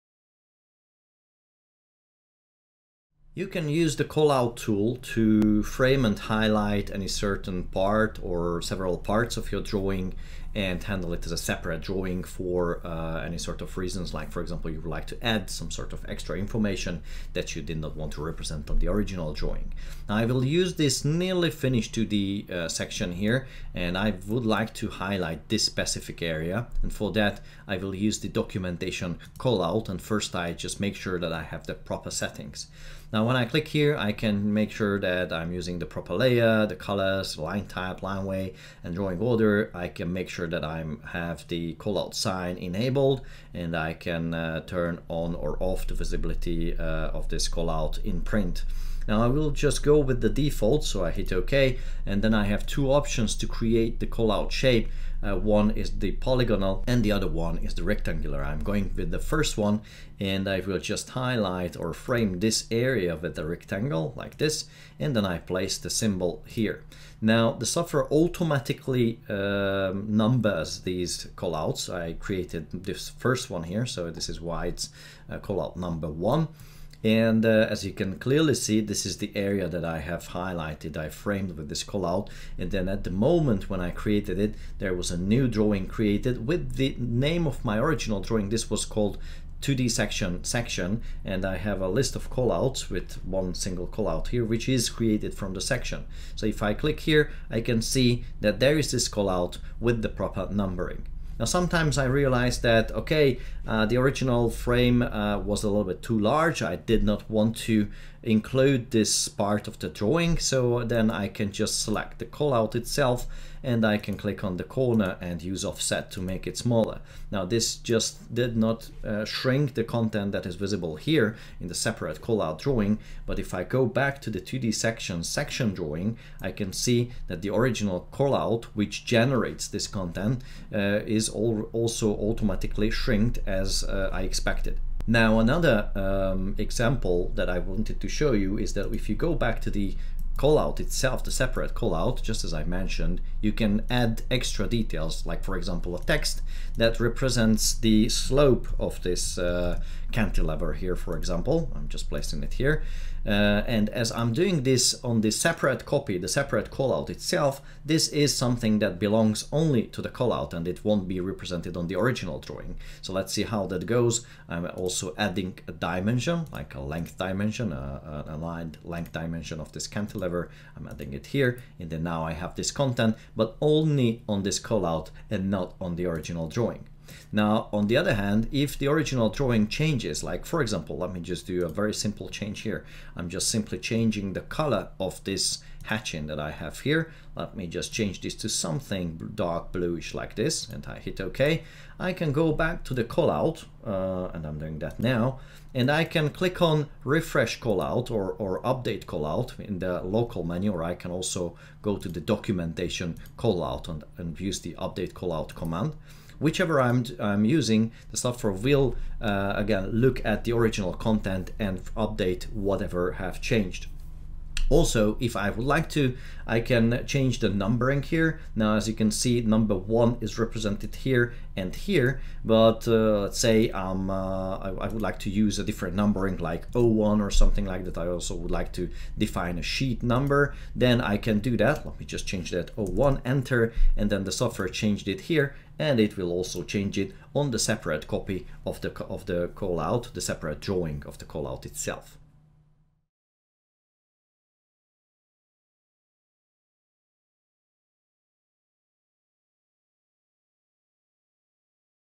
Working with callouts - ARCHLine.XP
Using callouts in ARCHLine.XP you can highlight a certain part of your drawing and define specific annotations and dimensions to this particular separate callout drawing. The callout can also be updated once the original drawing is changed.